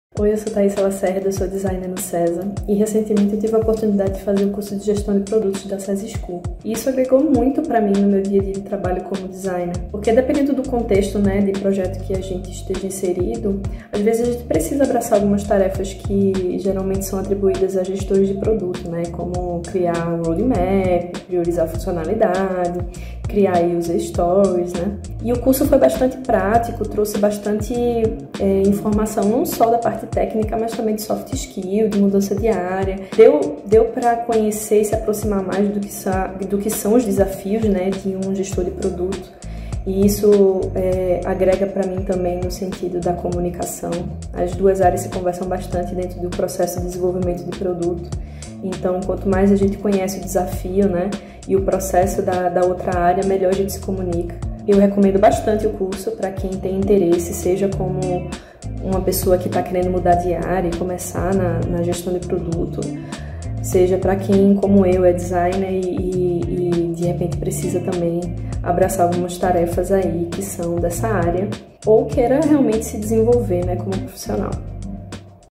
Depoimento da Thayssa, que é Designer e aluna do curso de Gestão de Produtos
Faça sua migração de carreira para Gestão de Produtos em um curso de 48h de aulas ao vivo, com conteúdos e práticas pensadas em quem está iniciando ou deseja iniciar na área! Clique e conheça os diferenciais do curso de Gestão de Produtos Digitais da CESAR School!